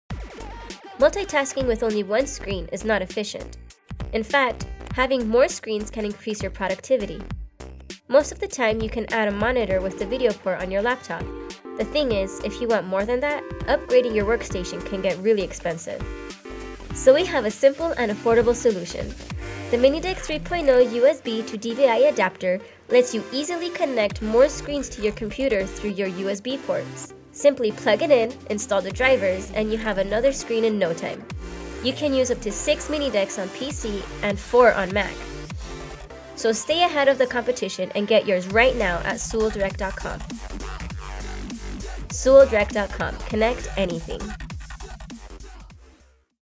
Sewell Minideck 3.0 - Add multiple monitors to your Mac or PC
UPDATE: Now available in USB-HDMI and USB-DisplayPort!
Easily add an extra monitor to your computer and enjoy the added graphics performance of USB 3.0 through a single USB port using Sewell's Minideck 3.0 USB to DVI Adapter. Perfect for presentations and adding screen real estate to improve productivity at work and play.

Product Features

DL 3.0 Video-Tuned Compression
Resolutions up to 2048 x 1152
supports full 1080p
Integrated DVI (with HDCP)
USB 3.0
Backwards compatible with USB 2.0
Protected video playback via HDCP 2.0
Sleek design and small footprint
Magnetic, allowing you to easily put it in an out-of-the-way place or stack multiple units
Drivers on flash drive for quick and easy installation
The Minideck 3.0 acts as an external graphics card, allowing you to use multiple high end monitors with standard PCs and laptops. With an upgraded chipset and new USB 3.0 technology, the Minideck 3.0 offers the best USB graphics available.